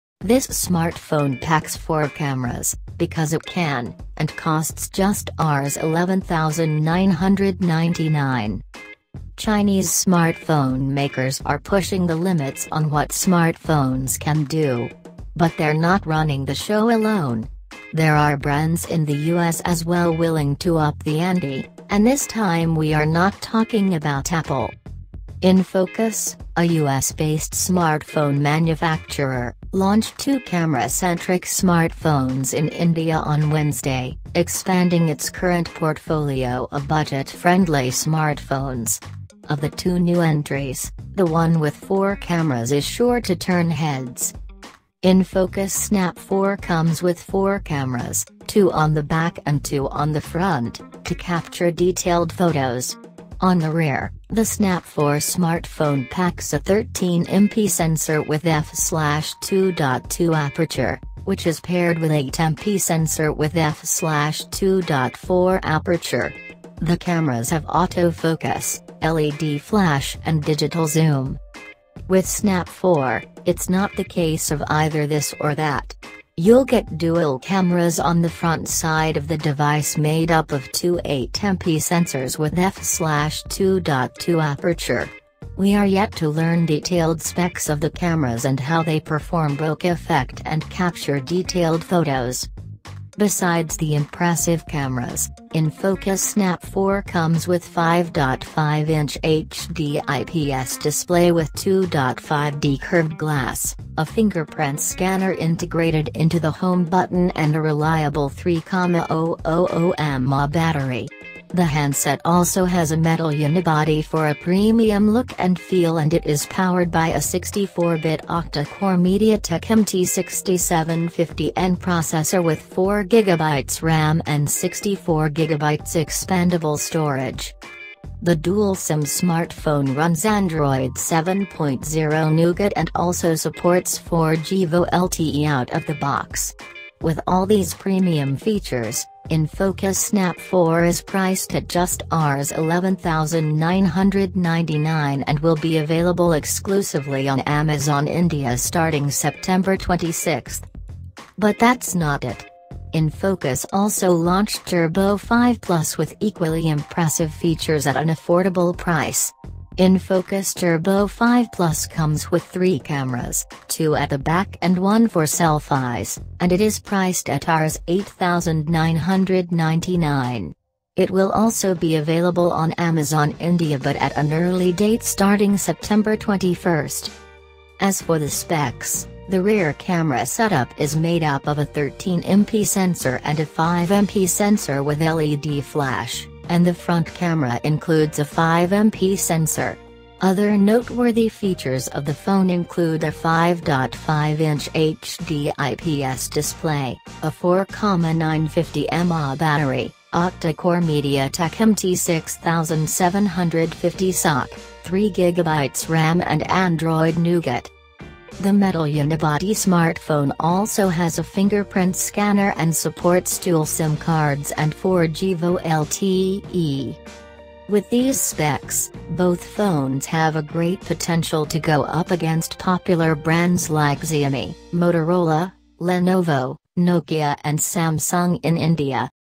Infocus snap 4 with 13 mp dual rear camera and dual front 8mp selfie camera for 11999/-. everything
InFocus Snap 4 comes with four cameras, two on the back and two on the front, to capture detailed photos. On the rear, the Snap 4 smartphone packs a 13MP sensor with f/2.2 aperture, which is paired with 8MP sensor with f/2.4 aperture.
 The cameras have auto focus, LED flash and digital zoom. With Snap 4, it's not the case of either this or that. You'll get dual cameras on the front side of the device made up of two 8MP sensors with f/2.2 aperture. We are yet to learn detailed specs of the cameras and how they perform Bokeh effect and capture detailed photos. Besides the impressive cameras,
InFocus Snap 4 comes with 5.5-inch HD IPS display with 2.5D curved glass, a fingerprint scanner integrated into the home button and a reliable 3,000mAh battery.
The handset also has a metal unibody for a premium look and feel and it is powered by a 64-bit octa-core MediaTek MT6750N processor with 4GB RAM and 64GB expandable storage. The dual SIM smartphone runs Android 7.0 Nougat and also supports 4G VoLTE out-of-the-box. With all these premium features,
InFocus Snap 4 is priced at just Rs 11,999 and will be available exclusively on Amazon India starting September 26.
But that's not it. InFocus also launched Turbo 5 Plus with equally impressive features at an affordable price.
InFocus Turbo 5 Plus comes with three cameras, two at the back and one for selfies, and it is priced at Rs 8,999.

Everything viral